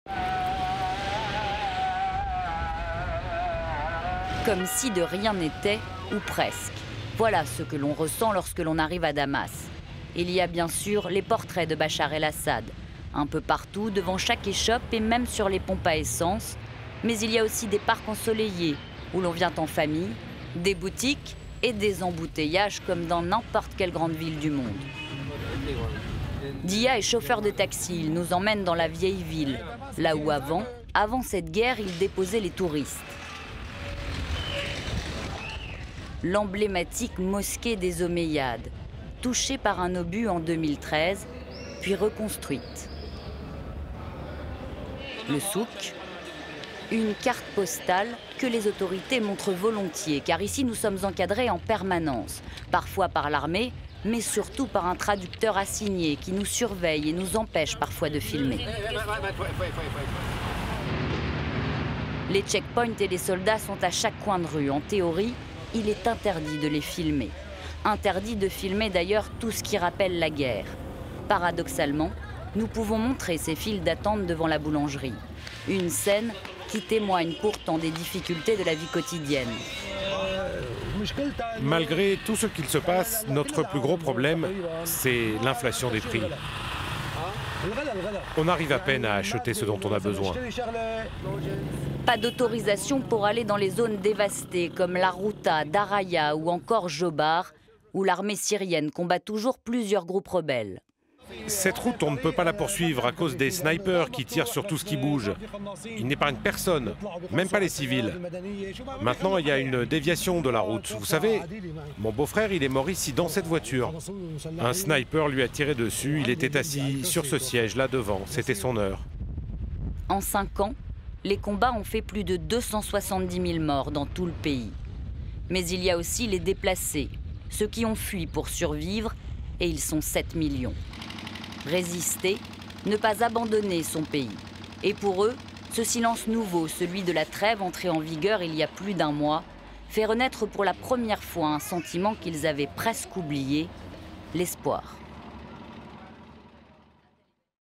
Syrie: à Damas, comme si de rien n'était, ou presque
Des parcs ensoleillés au souk de Damas, des boutiques aux embouteillages, la capitale syrienne semble vivre comme n'importe quelle ville dans le monde. Il y a aussi les soldats et les checkpoints, présents à chaque coin de rue, ainsi que les files d'attentes de syriens pour acheter à manger, qui rappellent la guerre en cours dans le pays. Depuis 5 ans le conflit syrien a fait plus de 270.000 morts dans le pays et 7 millions de déplacés.